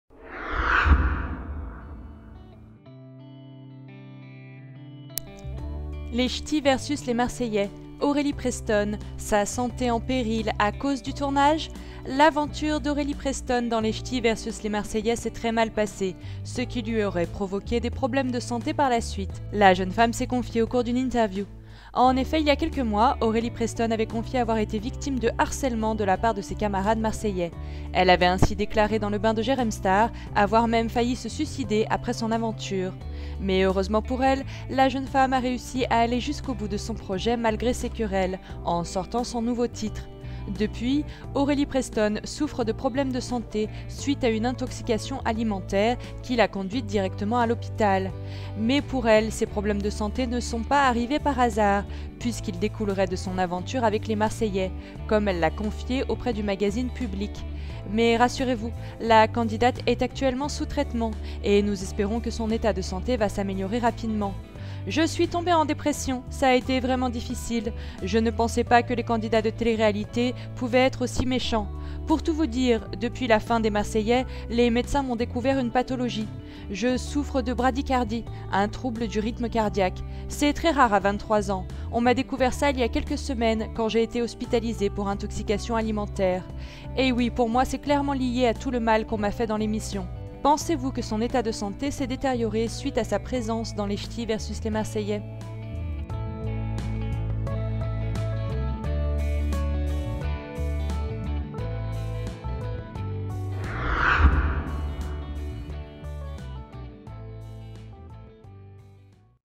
LCVSLM : Aurélie Preston, sa santé en péril à cause du tournage ?
L'aventure d'Aurélie Preston dans Les Ch'tis VS Les Marseillais s'est très mal passée, ce qui lui aurait provoqué des problèmes de santé par la suite. La jeune femme s'est confiée au cours d'une interview...